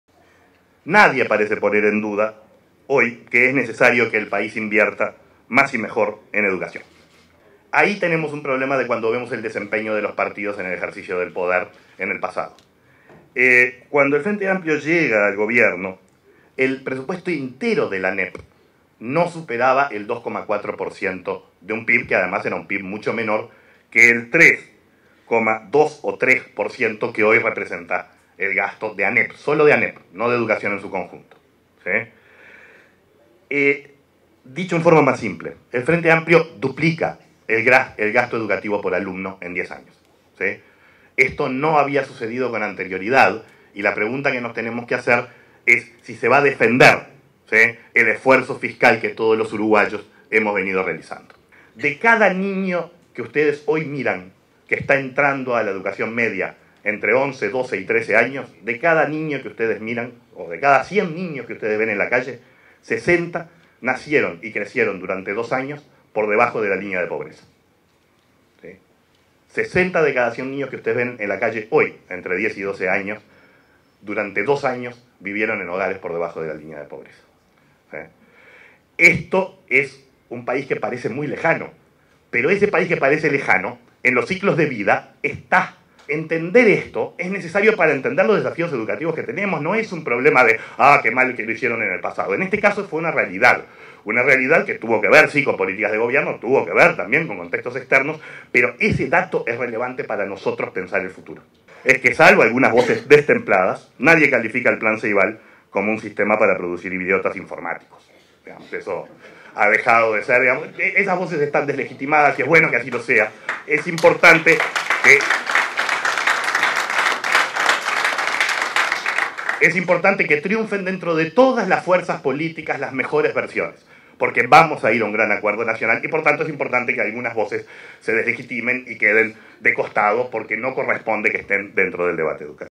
Fernando Filgueira habla en serio sobre educación
Técnicos de diferentes disciplinas hablan sobre el presente y futuro de Uruguay.